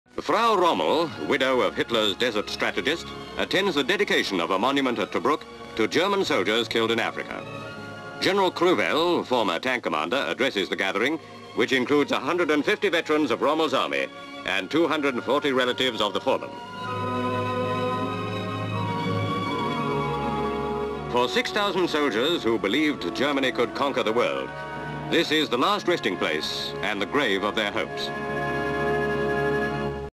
German Memorial (1955)
Tobruk, Libya, North Africa.

CU. Pan with Frau Rommel, a widow of the Hitler's famous General Rommel, walking to Tobruk monument. GV. Tobruk monument. GV. Crowds listening. CU. Former General of the tank corps, Gruewell. CU. People listening. GV. Interior, Monument. GV. Interior, monument, showing pylon with flames burning inside it. CU. Name plaque. CU. Woman laying a bunch of flowers on base of name plaque. SCU. Party of nuns watching. SC. Wreaths and bunches of flowers on base of name plaque.

(Lav.) (Orig.A.)
 FILM ID:551.33